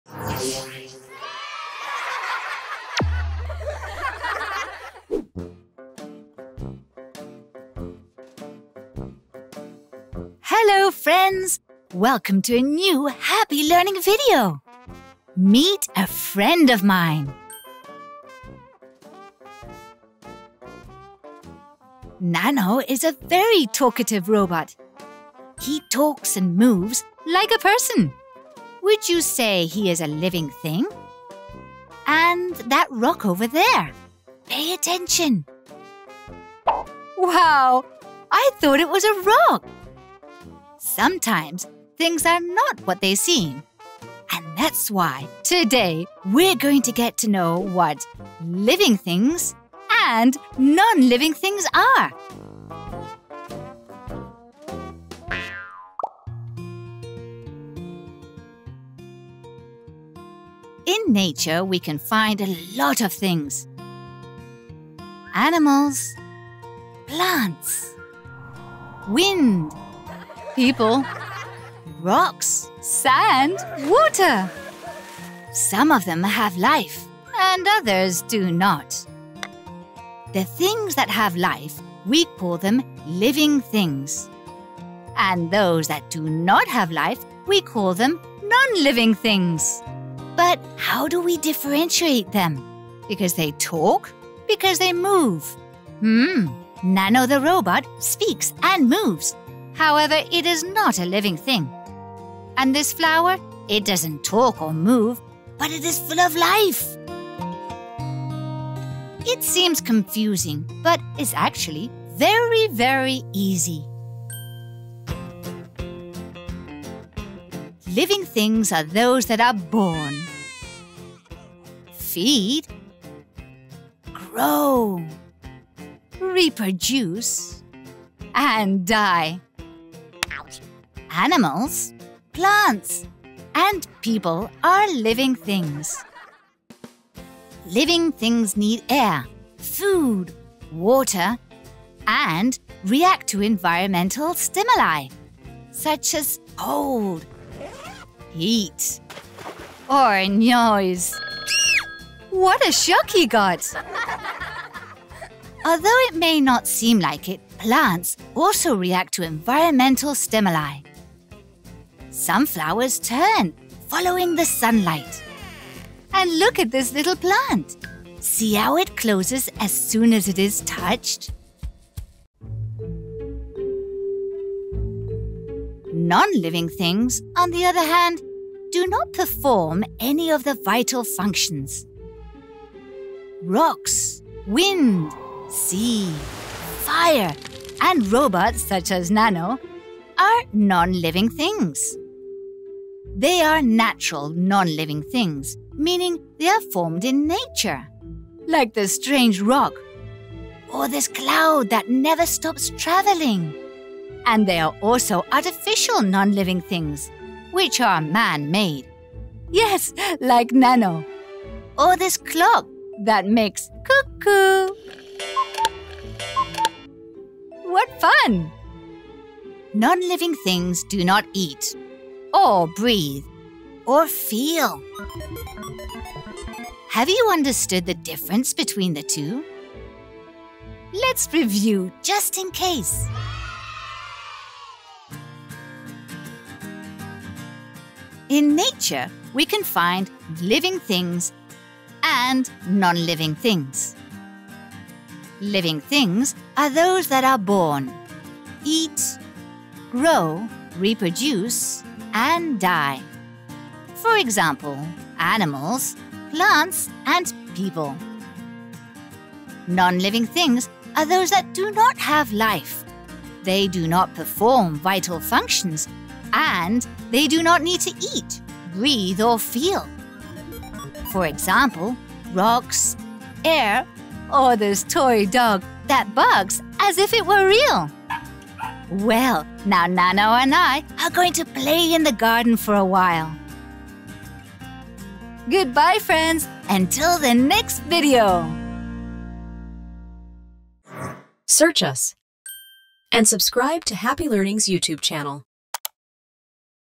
LIVING AND NON-LIVING THINGS | CURIOSITIES FOR KIDS | HAPPY LEARNING 🙉🌼 🤖🦾
Hello, Happy Learners! 🎉🌱 Welcome to a new video from Happy Learning. Today we are going to explore the differences between **living and non-living things**. 🌿✨ Yes, we are going to discover what makes something alive and what characteristics non-living objects have! 🌟😊

Did you know that living things can grow, reproduce, and respond to their environment? 🤔🌍🔍 Join us on this educational adventure and learn fascinating facts about the world around us. It will be a journey full of discoveries and fun! 😄🎉

Don't miss it! Let's learn and have fun together! 🚀😊

Discover hundreds of unseen resources! and learn in the most fun way.